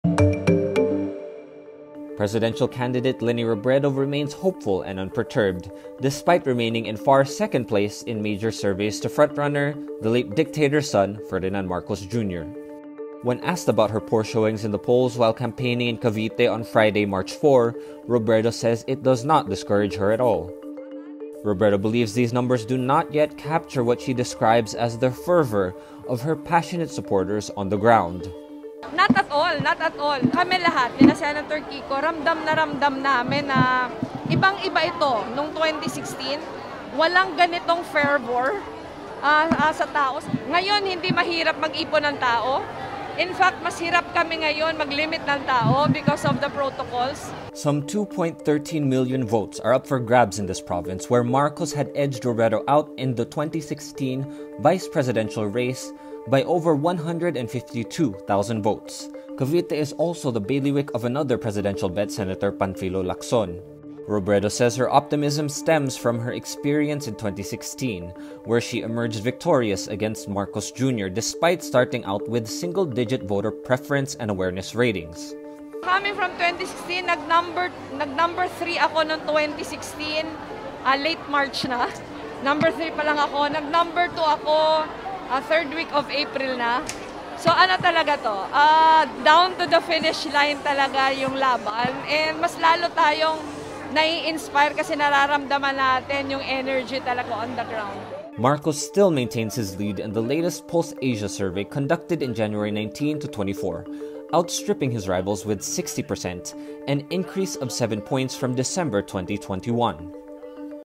Robredo ‘not at all’ discouraged by surveys: ‘This is a fight down to the finish line’
More videos on Rappler Presidential candidate Leni Robredo remains hopeful and unperturbed despite remaining in far second place in major surveys to frontrunner, the late dictator’s son Ferdinand Marcos Jr.

Full story Follow Rappler for the latest news in the Philippines and around the world.